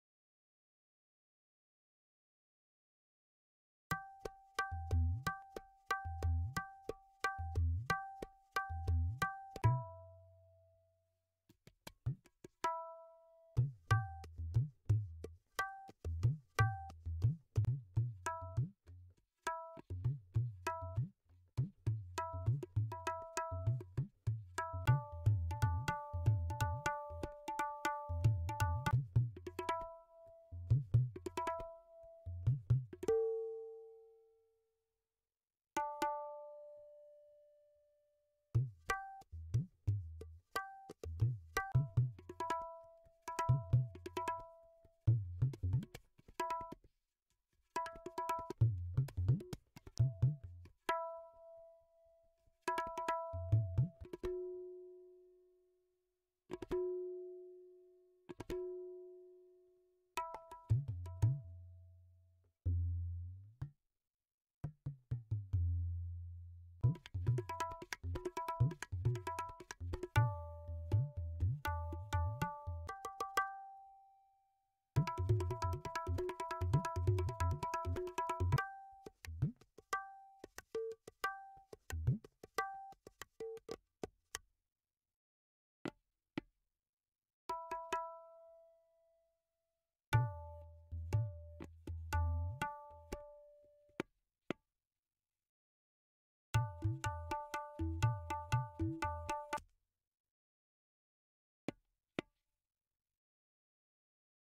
Steinberg | VST Loops and Sounds | Tabla
An eclectic collection of Indian tabla rhythms, recorded and produced in Mumbai, India, offers a selection of all worlds: Bollywood, Traditional and Experimental. From slow and airy to busy and expressive beats, a perfect addition for your production in very high quality.

• Recorded in 24 bit and 96 kHz, downsampled to 44.1 kHz •174 loops and 307 one shots • 738 MB • Tempos: 40 BPM-135 BPM • Played by Vinayak Netke